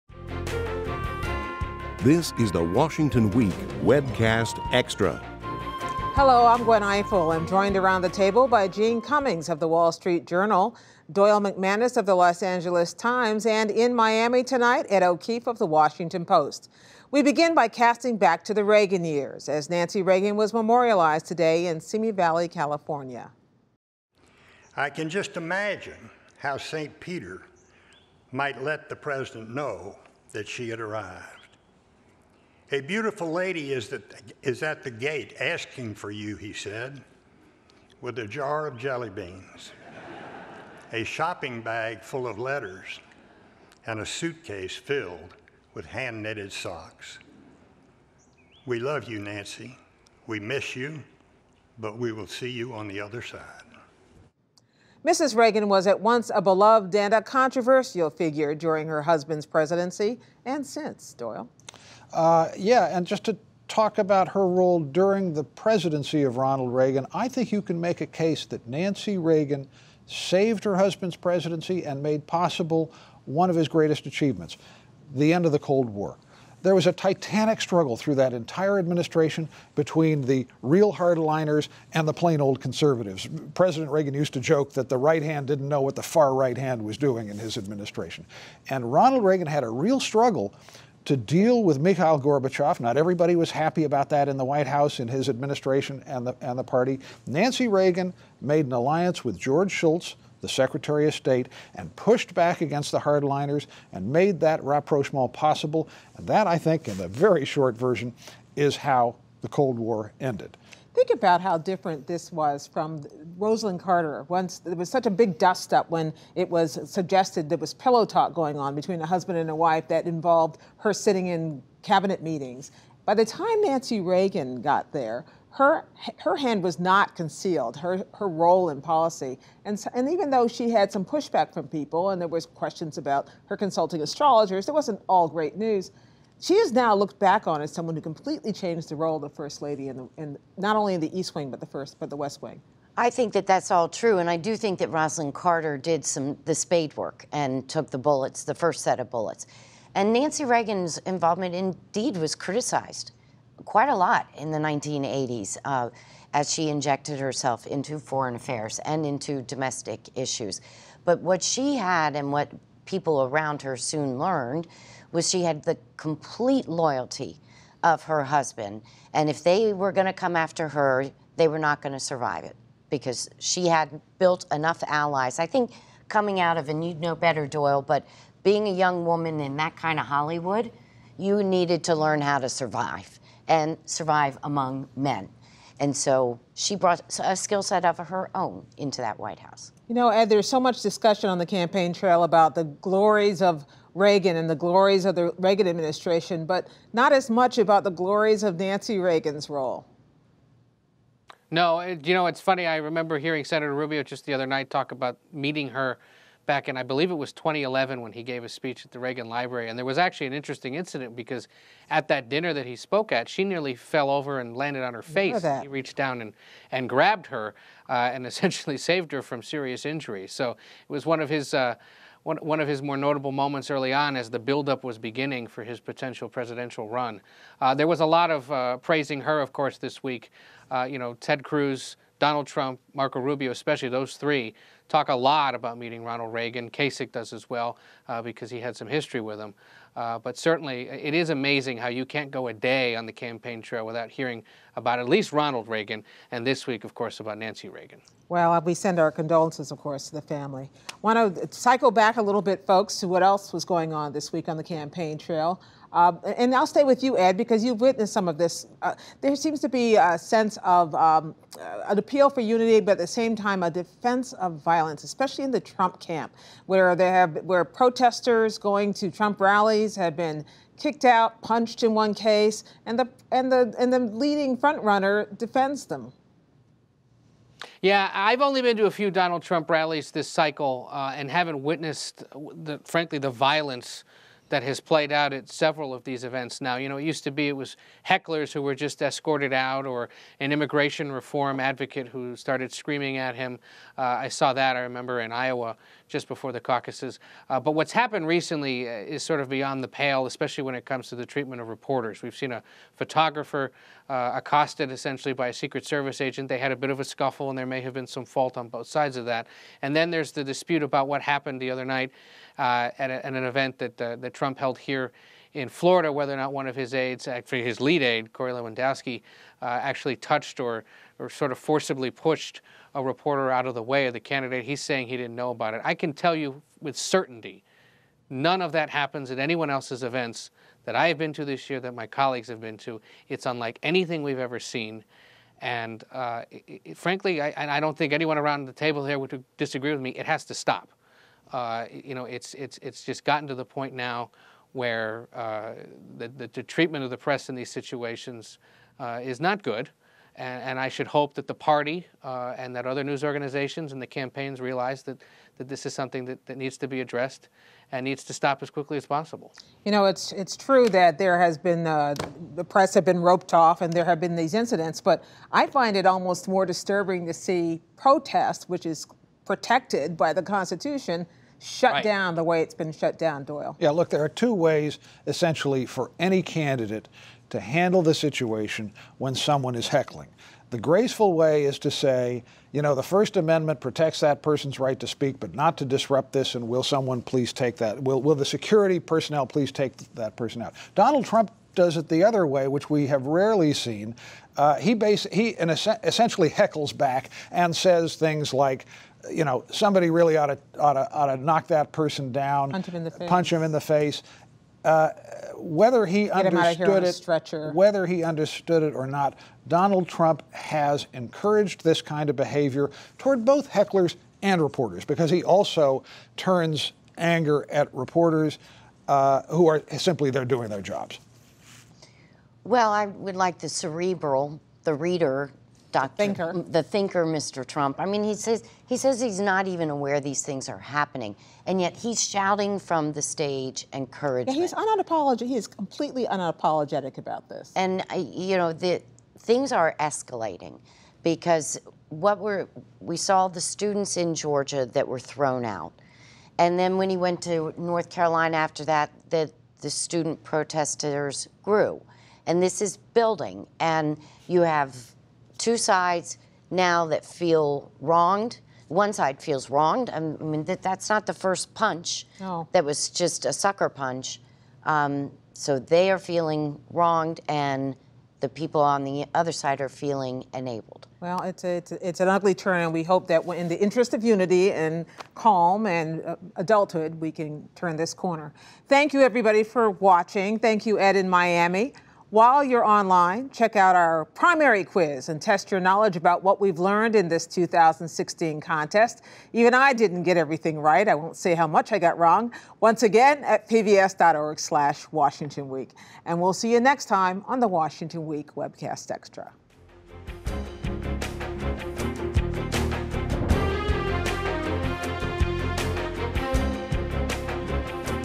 Remembering Nancy Reagan and the Role of Protesters on the 2016 Campaign Trail
Former First Lady Nancy Reagan was remembered today during a funeral service at Ronald Reagan's Presidential Library in Simi Valley, California.  Reagan, who died last week at 94, was a true partner for her husband during his administration and the final years of the Cold War.  Meanwhile, the Republican candidates are trying to carry the Reagan legacy in this year's presidential election. Rallies for frontrunner Donald Trump have taken a noticeably violent turn in recent weeks.  One protester was punched as he was led out of the stadium, and several reporters have had dust-ups with Trump's team. While Trump has condoned violence, Doyle McManus says the tone of his events encourages the behavior.